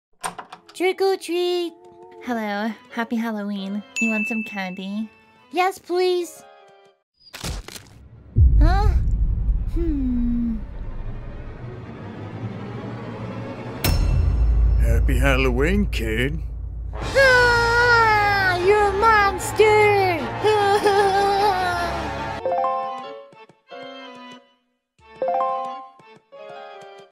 Trick or Treat Animation
A Halloween animation is about you the viewer getting a "Trick" or "Treat".

I know I'm gonna get comments telling me that this animation was late. Yes I know but animation takes too long... haha